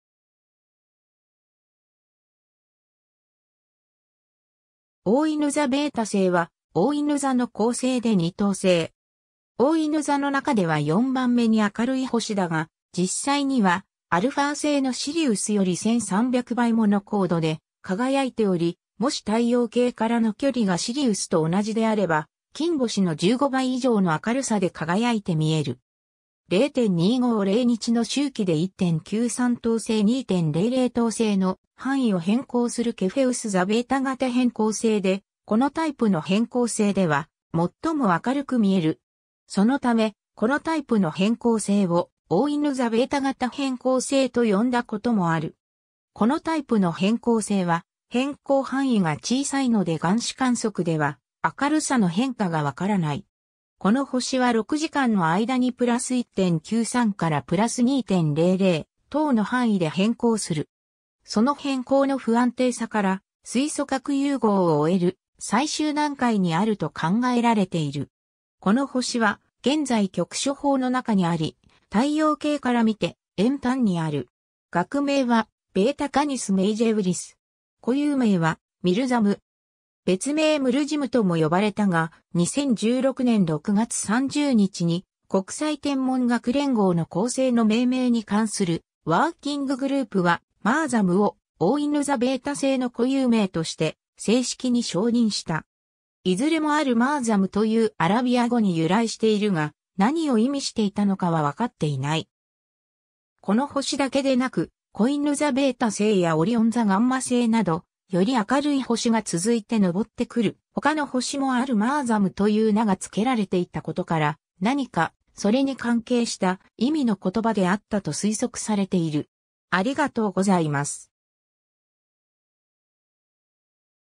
おおいぬ座ベータ星
おおいぬ座β星は、おおいぬ座の恒星で2等星。
 おおいぬ座の中では4番目に明るい星だが、実際にはα星のシリウスより1,300倍もの光度で輝いており、もし太陽系からの距離がシリウスと同じであれば金星の15倍以上の明るさで輝いて見える。
 0.250日の周期で1.93等星～2.00等星の範囲を変光するケフェウス座β型変光星で、このタイプの変光星では最も明るく見える。
そのため、このタイプの変光星を、「おおいぬ座β型変光星」と呼んだこともある。
このタイプの変光星は変光範囲が小さいので眼視観測では明るさの変化がわからない。
この星は6時間の間に+1.93から+2.00等の範囲で変光する。
その変光の不安定さから、水素核融合を終える最終段階にあると考えられている。
 この星は現在局所泡の中にあり、太陽系から見て遠端にある。
 学名はβ Canis Majoris（略称はβ CMa）。
固有名はミルザム (Mirzam) 。
別名ムルジム（Murzim）とも呼ばれたが、2016年6月30日に国際天文学連合の恒星の命名に関するワーキンググループ (Working Group on Star Names, WGSN) は、Mirzam をおおいぬ座β星の固有名として正式に承認した。
いずれも al-mirzam というアラビア語に由来しているが、何を意味していたのかはわかっていない。
この星だけでなく、こいぬ座β星やオリオン座γ星など、より明るい星が続いて上ってくる他の星も al-mirzam という名が付けられていたことから、何かそれに関係した意味の言葉であったと推測されている。